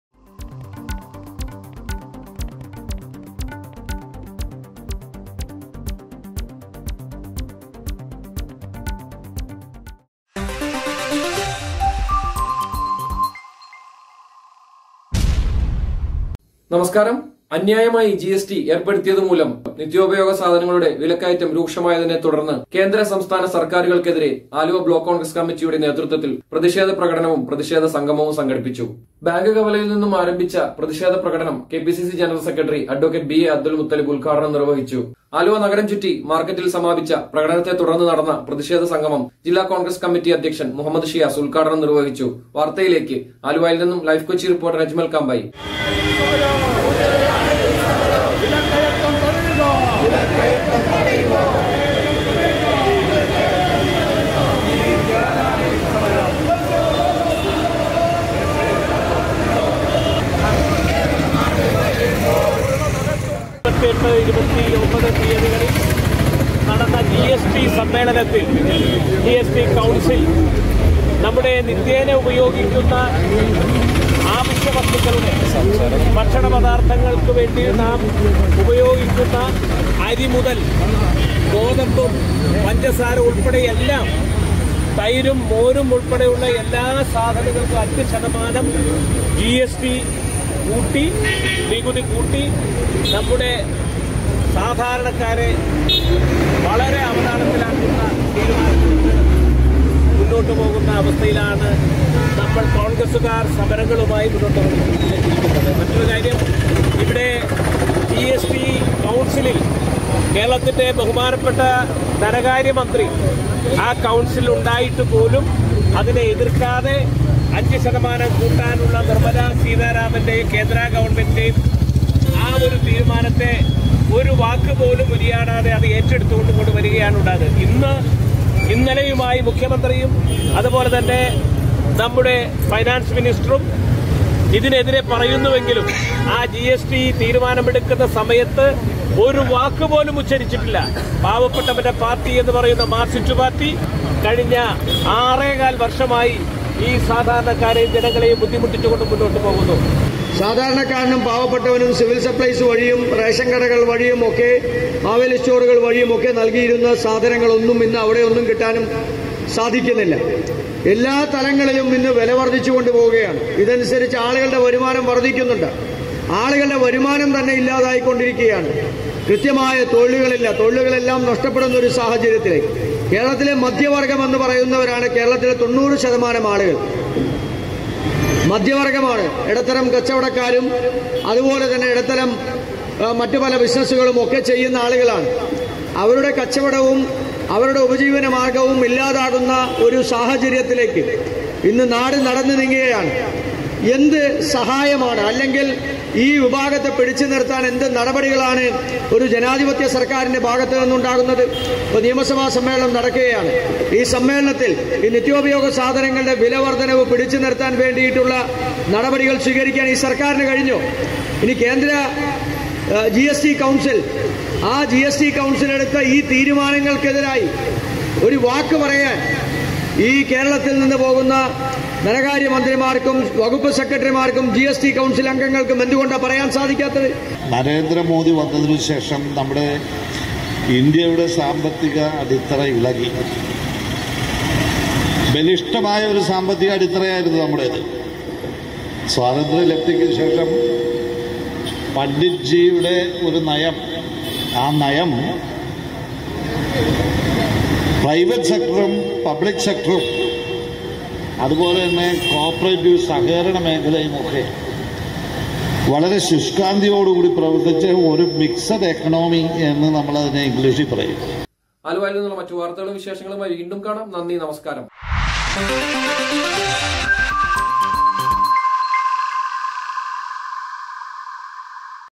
വിലവർദ്ധനവിനെതിരെ ബ്ലോക്ക് കോൺഗ്രസ്സ് കമ്മിറ്റിയുടെ നേതൃത്വത്തിൽ പ്രതിക്ഷേധ പ്രകടനവും സംഗമവും നടന്നു
ആലുവ : ബാങ്ക് കവലയിൽ നിന്നും ആരംഭിച്ച  പ്രതിക്ഷേധ പ്രകടനം KPCC ജനറൽ സെക്രട്ടറി അഡ്വ. ബി.എ.അബ്ദുൽ മുത്തലിബ് ഉദ്‌ഘാടനം നിർവഹിച്ചു. നഗരം ചുറ്റി മാർക്കറ്റിൽ സമാപിച്ച പ്രകടനത്തെ തുടർന്ന് നടന്ന പ്രതിക്ഷേധ സംഗമം മുഹമ്മദ്‌ ഷിയാസ് പ്രസിഡന്റ്‌ ജില്ലാ കോൺഗ്രസ്‌ കമ്മിറ്റി ഉദ്‌ഘാടനം ചെയ്തു. എം ഒ ജോൺ അധ്യക്ഷൻ ആലുവ നഗരസഭ ചടങ്ങിൽ സംസാരിച്ചു. വാർത്തയുമായി ലൈഫ്കൊച്ചി റിപ്പോർട്ടർ അജ്മൽ കാമ്പായി